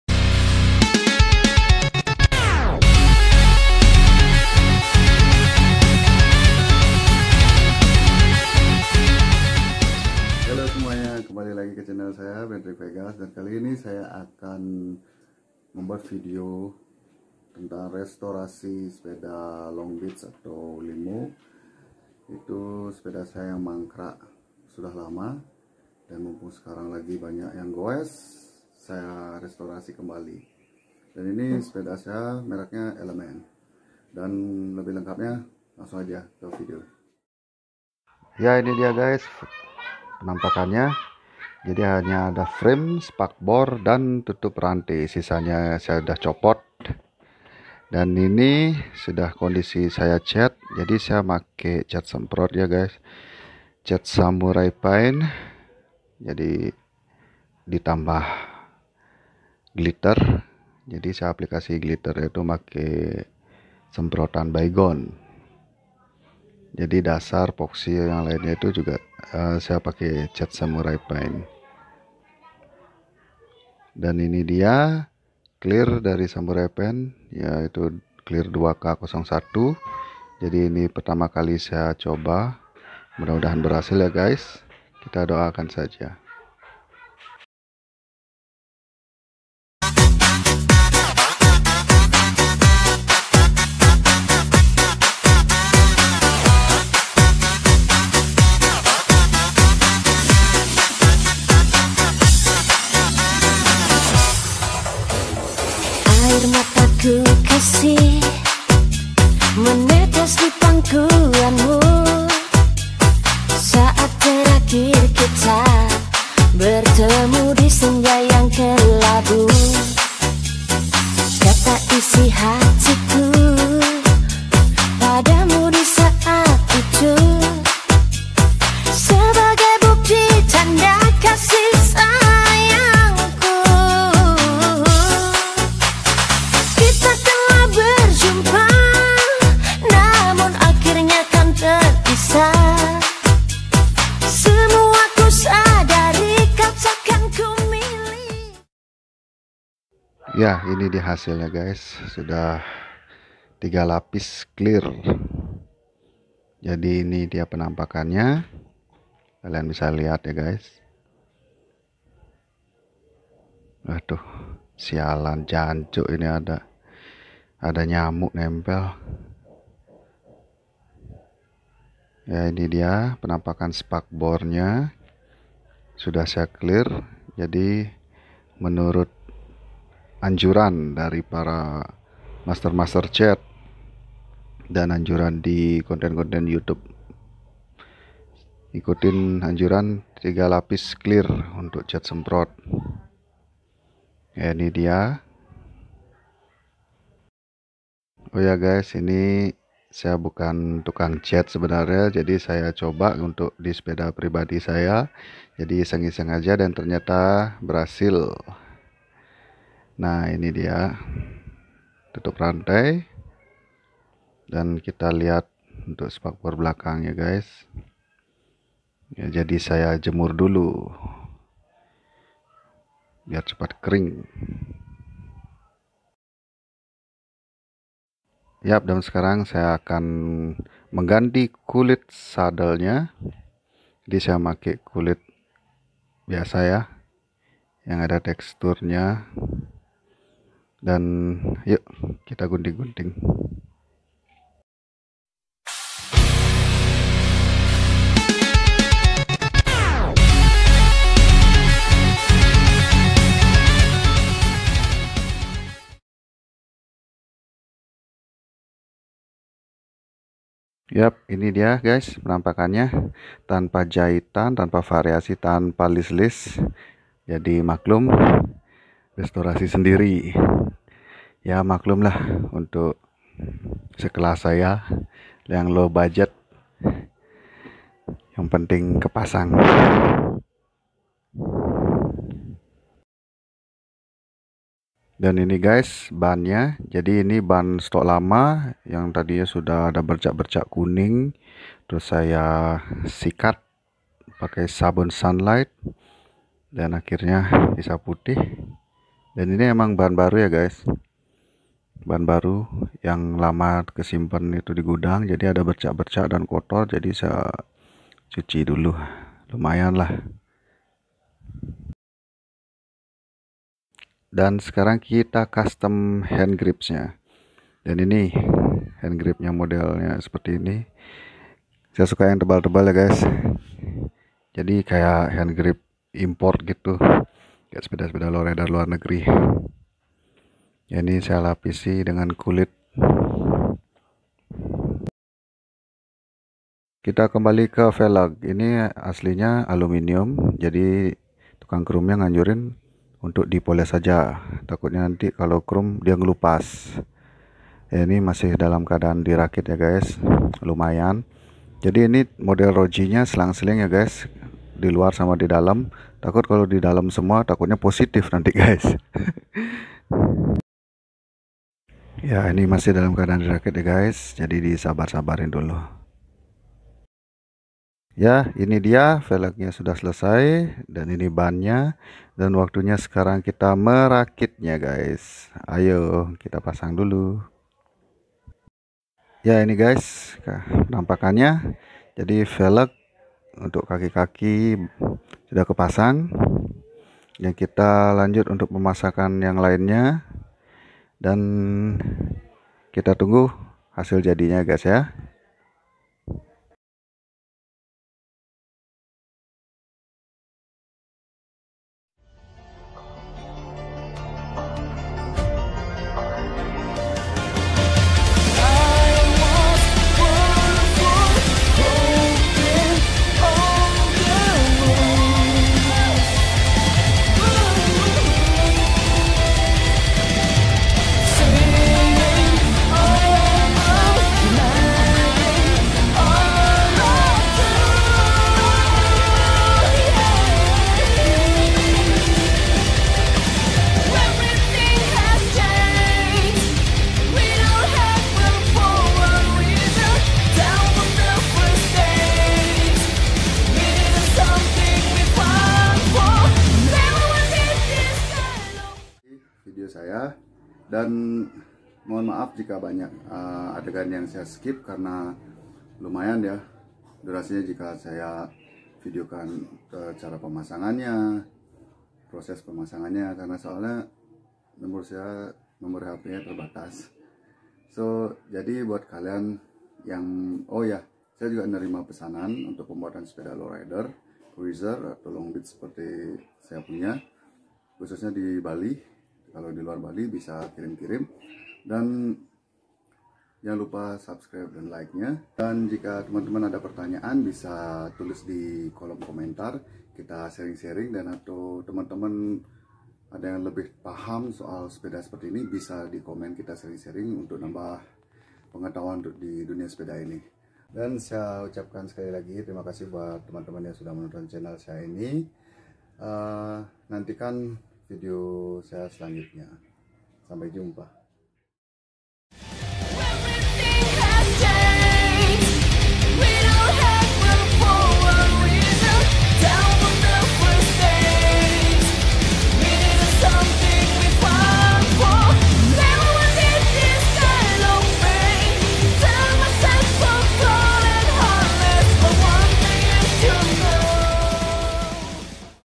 Restorasi sepeda element long beach/limo
Terima kasih buat teman2 yg sudah menonton video saya. Di video saya ini,saya mencoba untuk merestorasi sepeda milik pribadi saya. Kebetulan saya punya sepeda long beach merk Element yang lama mangkrak di gudang. Untuk selanjutnya,kalian bisa tonton video saya